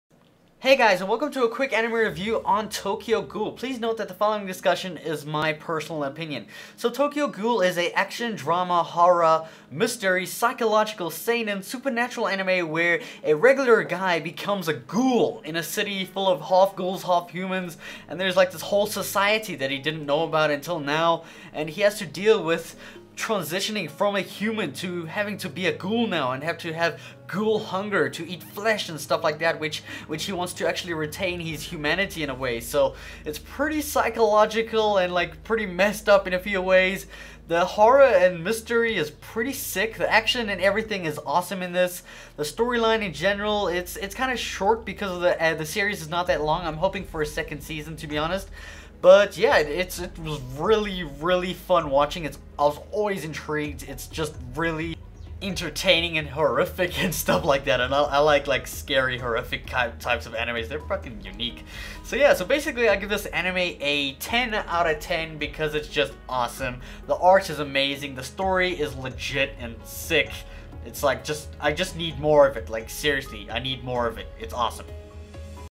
Quick Anime Review - Tokyo Ghoul
A personal quick review on "Tokyo Ghoul" which aired in 2014.

I give it 10 / 10.